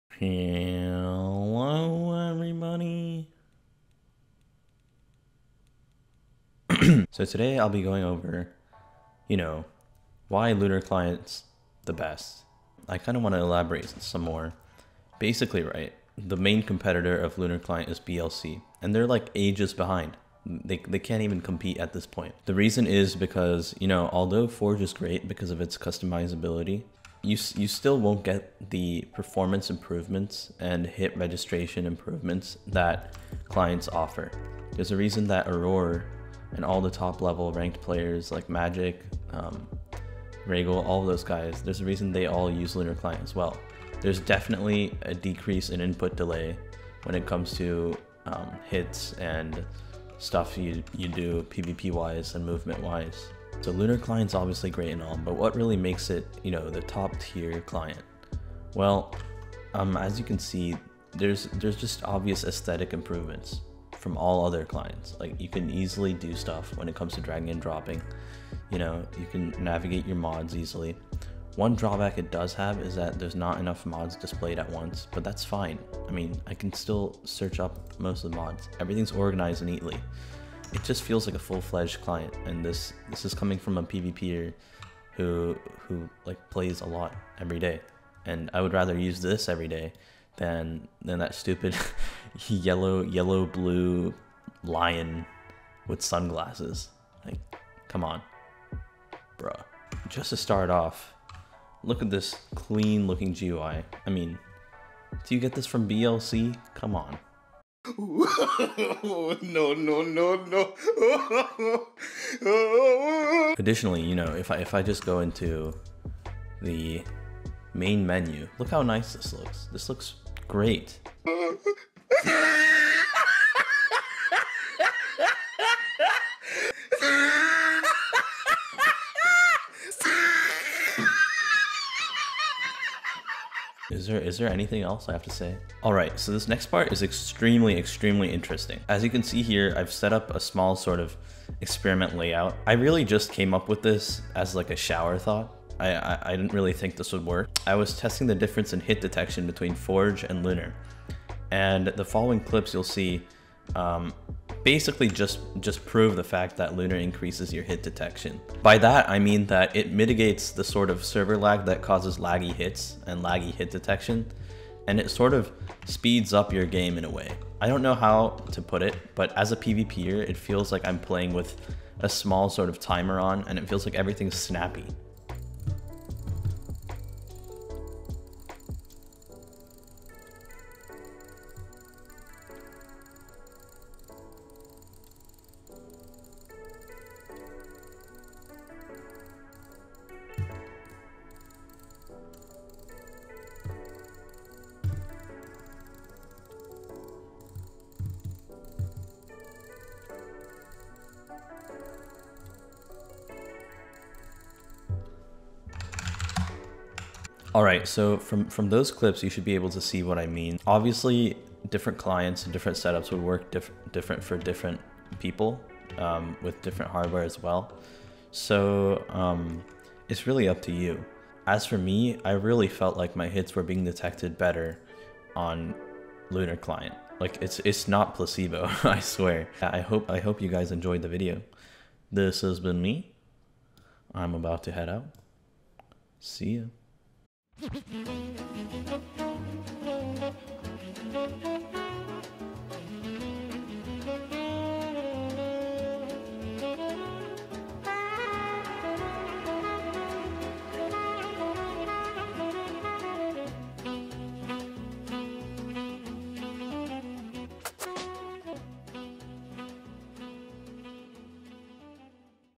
Why Lunar is the Best Client (Proof it gives better hits)
Yes ik clients can be optimized for different hardware and parts, so it really depends on your preference. I don't know why anyone would use BLC though, it looks like trash...